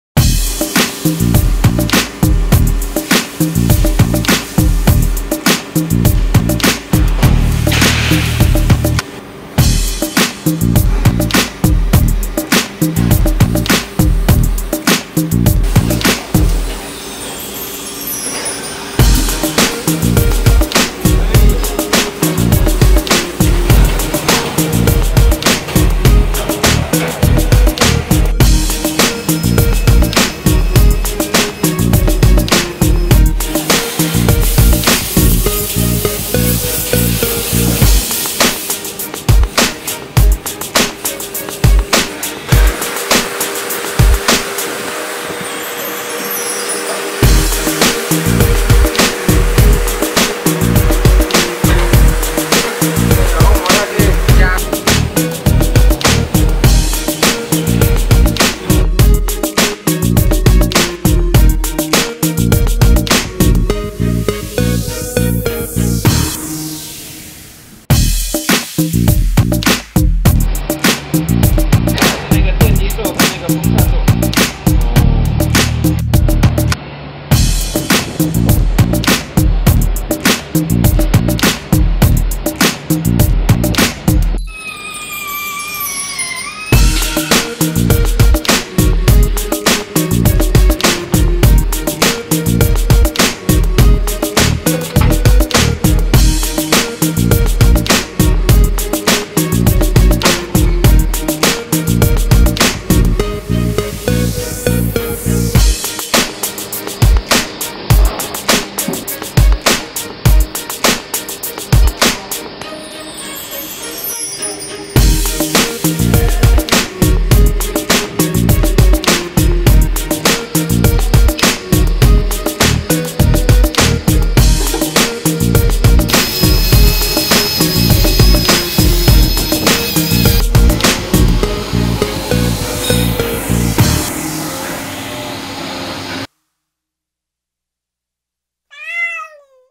How Aimicat is Made
At the end of September, we are finishing the molds. Here are some clips for the molds test by trial injection molding.